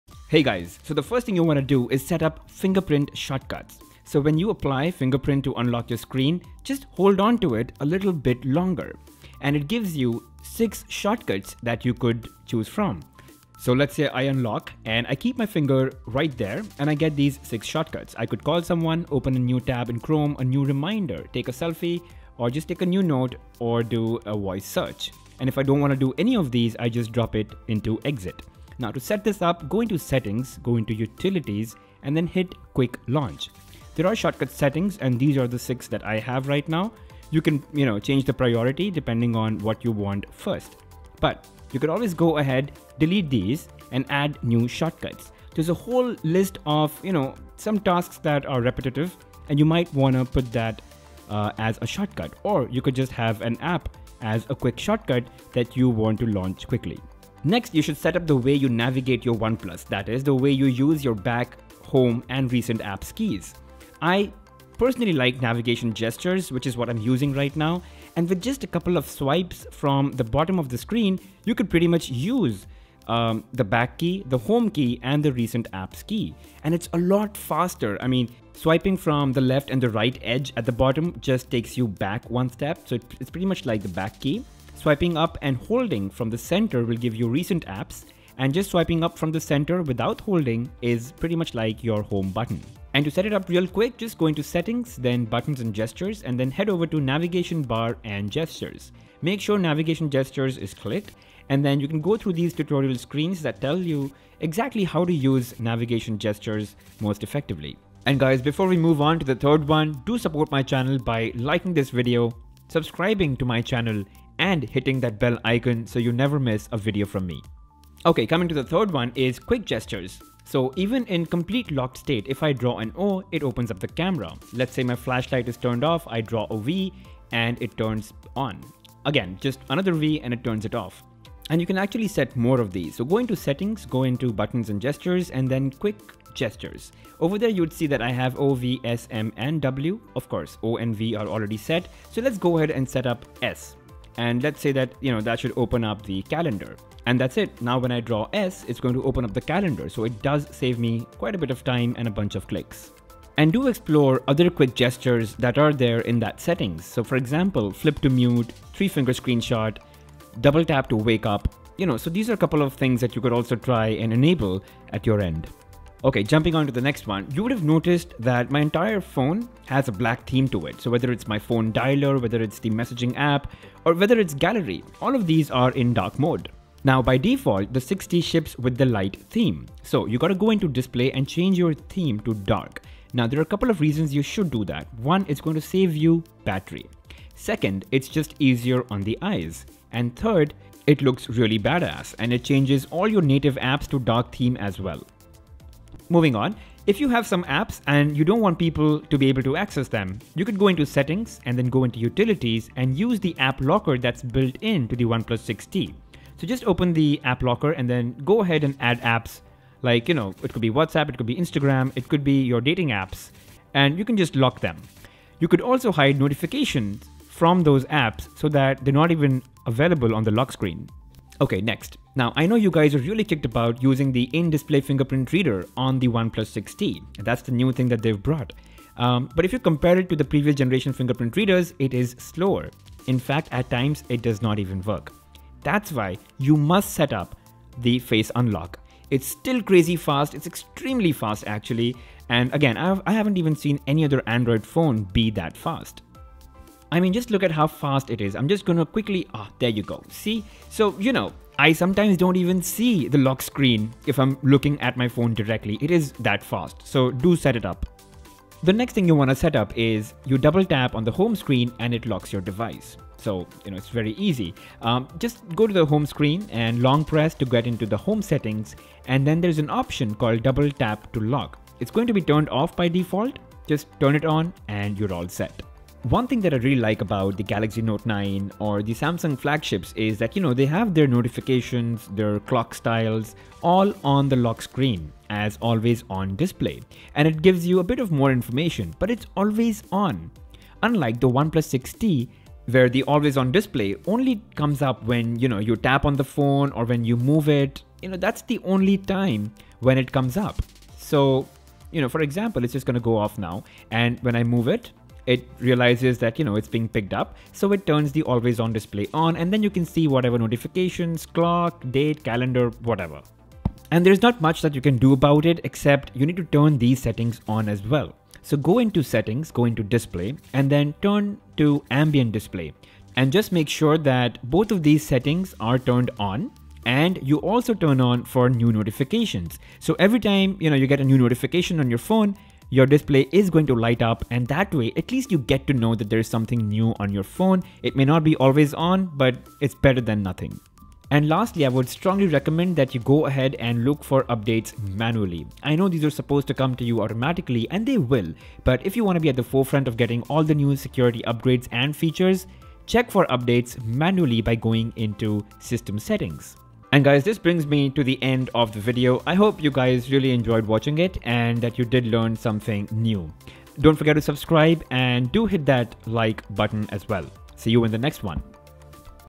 Top 10 MUST DO things to set up OnePlus 6T
Setup your Oneplus 6T for best performance, quick navigation and useful shortcuts. Tips and tricks to get your way with the OnePlus 6T.

Hacks that you’ve probably not explored yet.

Learn the hidden features of the OnePlus 6T.

Fingerprint Gesture/Shortcut
Fingerprint Gesture leads to quick shortcuts that you can configure at your end. Whether its calling someone, opening an app, taking a quick note, a quick selfie or putting a reminder - long press on the fingerprint sensor at the time of unlocking to access.

Navigation Gestures
Fastest way to navigate your OnePlus 6T is using swiping gestures to execute back, recent and home keys. Set it up within settings by going into Gestures and Buttons and then changing your navigation style to Navigation Gestures.

Quick Gestures
Draw on the locked screen to quickly launch camera or turn your flashlight on or open any other app that you like.

Change theme to Dark/Black
OnePlus 6T by default ships with light/white theme. Changing the theme to dark makes it look good, increases readability, puts lesser strain to the eyes and also conserves/saves more battery.
You can also apply your favourite accent color and make it look better.

App Locker
There is an in-built app locker that can protect your apps from being used by anyone else. Go into settings and then utilities and use the app locker setting to add apps that you would like to lock. those apps will then only open when you put in your pin. And you can hide notifications from those apps so no one can see them.

Double tap to lock
You can double tap anywhere on the homescreen to lock your phone. So you dont always have to use the lock-key to lock your phone. Sometimes, it is just a lot faster to double tap on a blank area on the homescreen to lock the phone. To enable, go to homescreen, long tap and click on Home Settings and enable 'Double tap to lock'.

Ambient Display
Turn on ambient display on the OnePlus 6T to see notifications/time/calendar every time you pick up the phone or move the phone or when a new notification comes in. To enable, go into settings, then display and choose 'Ambient Display.

Face lock
it is now the fastest way to unlock your oneplus 6T since the in-display fingerprint reader is not as fast anymore. So you must set up the Face unlock mechanism

Software update
Check your phone manually for any software updates that await. Though you do get them automatically, but check them manually if you want to be in the front of the queue.